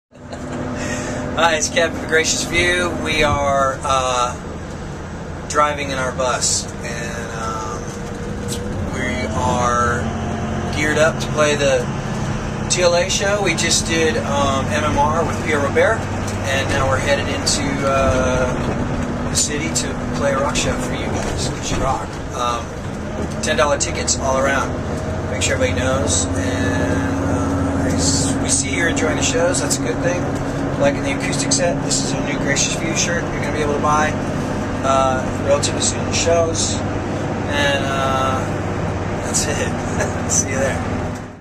The Gracious Few in Philadelphia at the TLA!!!
Kevin Martin sayin a quick hello to all of Philadelphia and letting you know about the show at the TLA, 10/01/10!!  Tickets are $10 at the door!!!